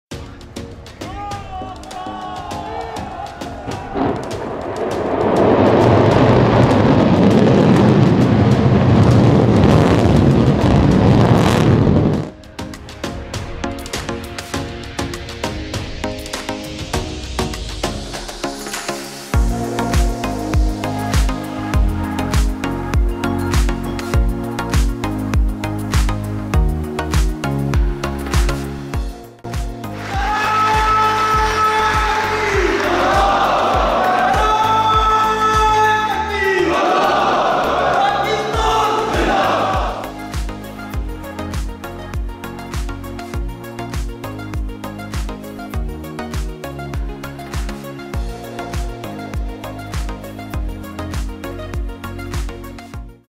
Pakistan's Shaheen 3 Ballistic Missile Test 2022 | Shaheen III Specifications | AM Raad
ISPR
Pakistan today (9 April 2021) conducted successful flight test of Shaheen-III surface to surface ballistic missile. The test flight was aimed at revalidating various design and technical parameters of the weapon system.